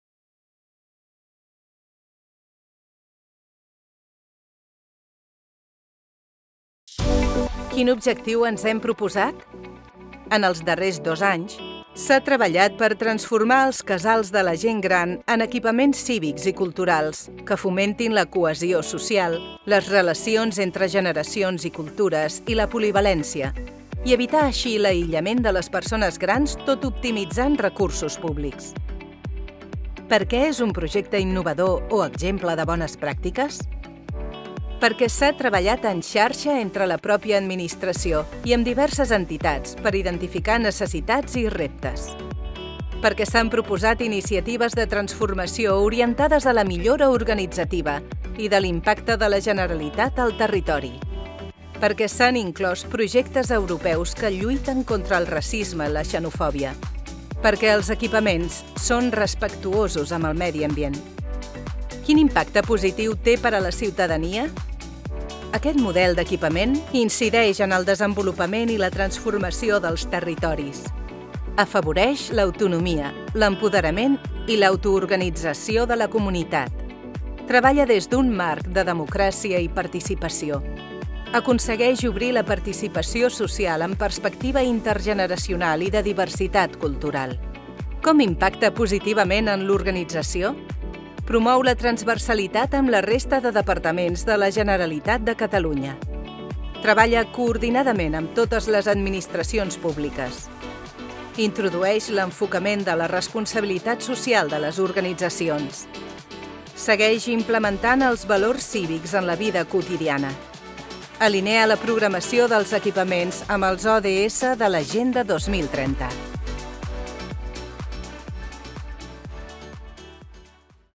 Culminació del procés de transformació dels casals de gent gran en equipaments cívics i comunitaris
Primer premi en la categoria 4 "Serveis finalistes" de la 6a edició dels Premis Alfons Ortuño 2022.